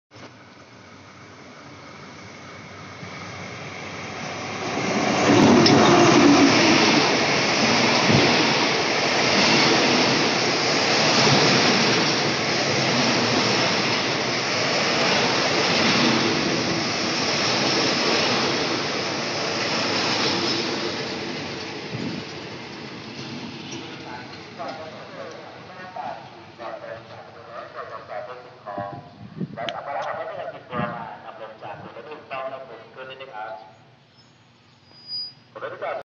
ขบวนรถเร็วที่102 เชียงใหม่ - กรุงเทพ กำลังเข้าสู่สถานีบ้านตาคลี
ALSTHOM (ALS)4116 ทำขบวนรถเร็วที่102 เชียงใหม่ - กรุงเทพ ขณะกำลังเข้าสู่สถานีบ้านตาคลี 05/07/63